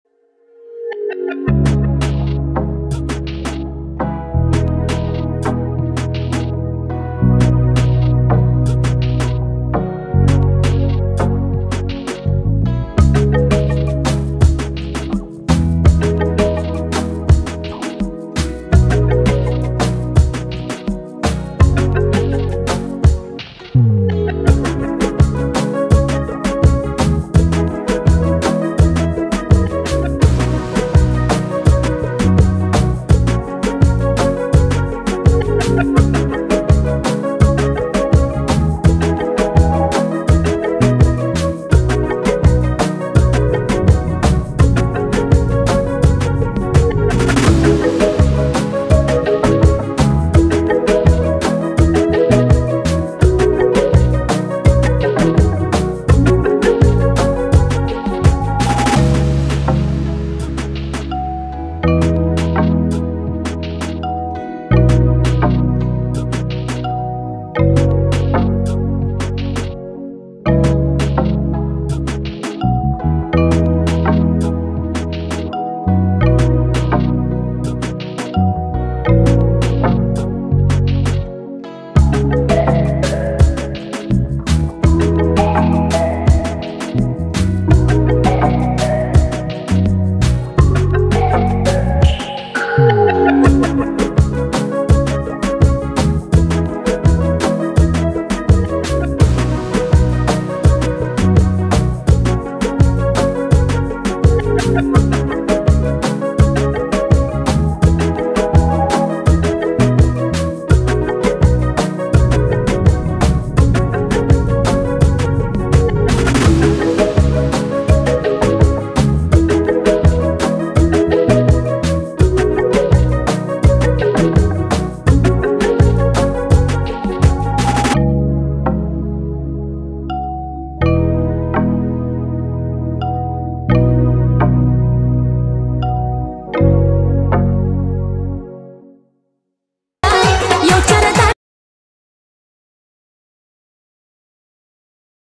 Urwa khan Student Hermani Dance2021/urwakhan
عروا خان کی شاگرد ہیرا مانی کا ایسا ڈانس کے دیھک کر آپ کے ہوش اور جائیں۔اک بار ضر دیکھیں